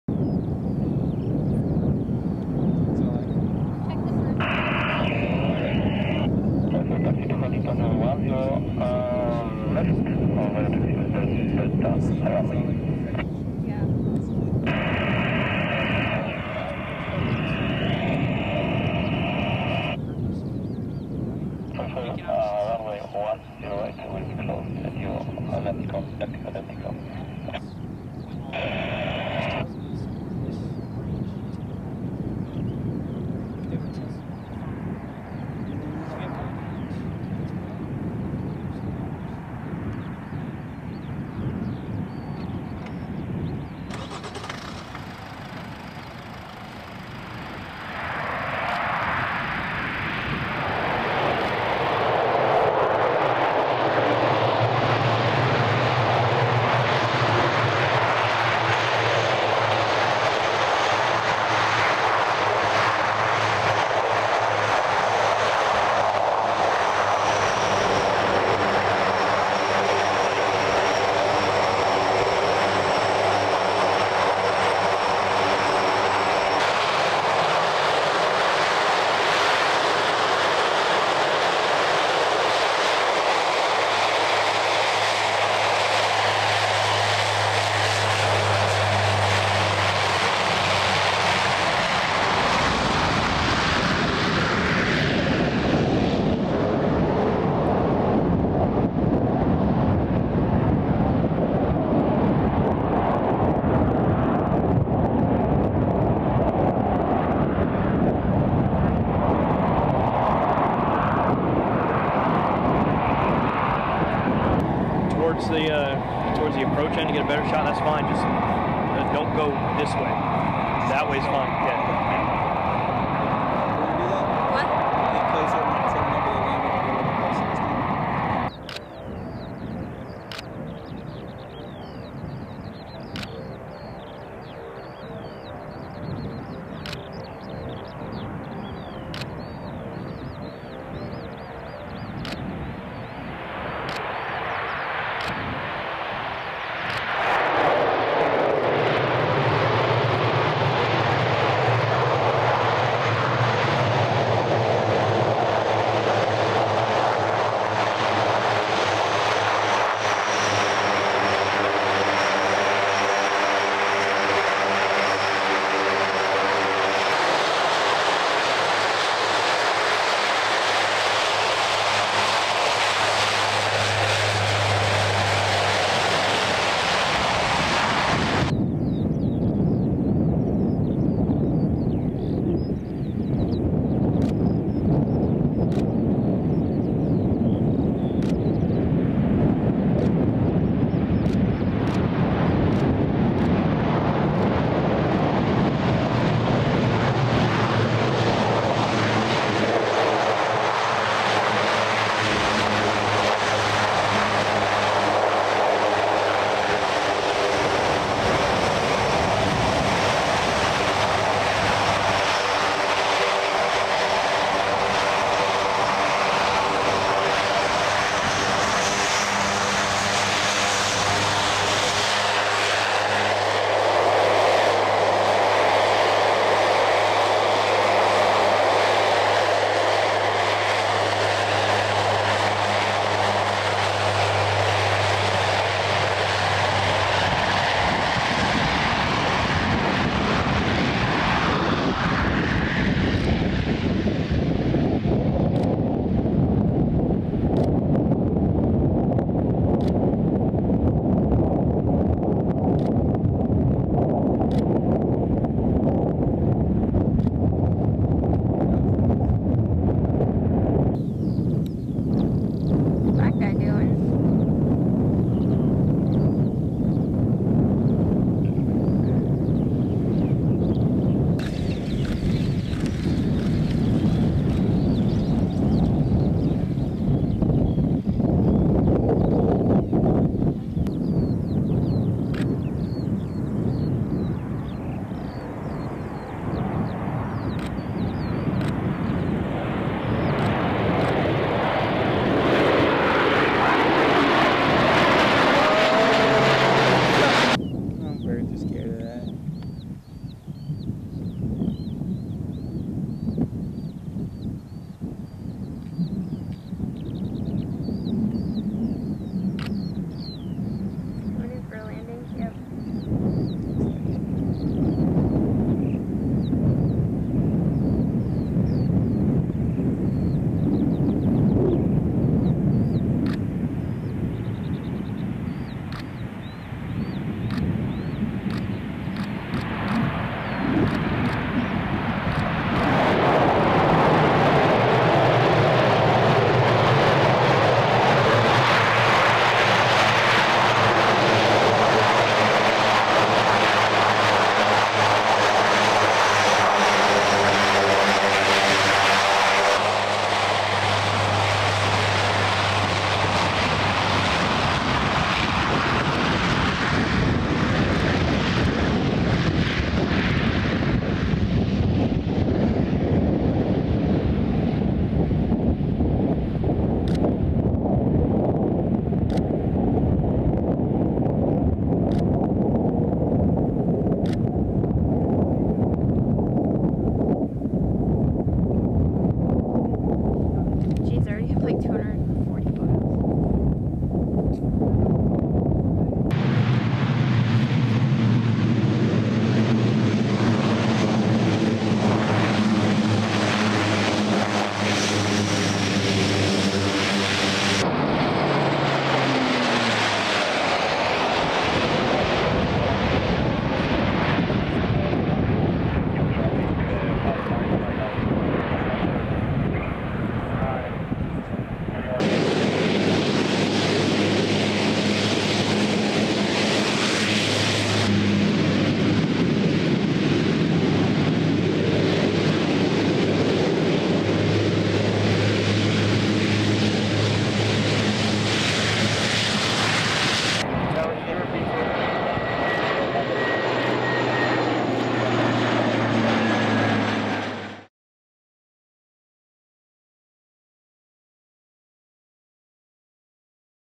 C-130 Grass Strip Landings
Video by Senior Airman Marvin Wigfall 86th Airlift Wing
Footage of 86th Airlift Wing C-130s landing on a grass strip in Poland.